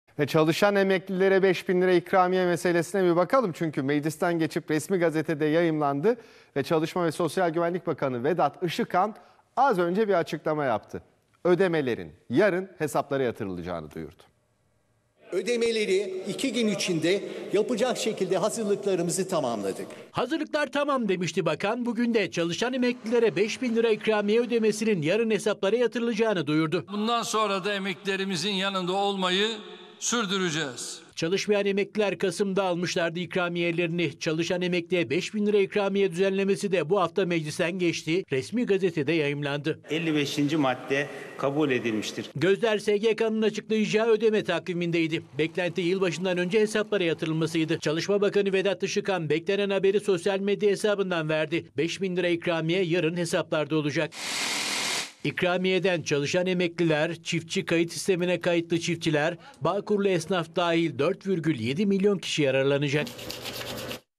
Bakan Vedat Işıkhan Duyurdu! Çalışan Emeklilere 5 Bin TL İkramiye Yarın Hesaplarda!
TBMM Genel Kurulu'nda, çalışan emekliye 5 bin TL ikramiye ödenmesine ilişkin düzenlemeyi de içeren teklif, kabul edilerek yasalaştı. Çalışan emeklilere 5 bin TL ikramiye verilmesi ile ekonomiye ilişkin düzenlemeleri içeren kanun, Resmi Gazete'de yayımlandı.
  Merakla beklenilen ödeme tarihine ilişkin Çalışma ve Sosyal Güvenlik Bakanı Vedat Işıkhan yaptığı açıklama ile 29 Aralık Cuma günü emekli ikramiyesi ödemelerinin yapılacağını duyurdu.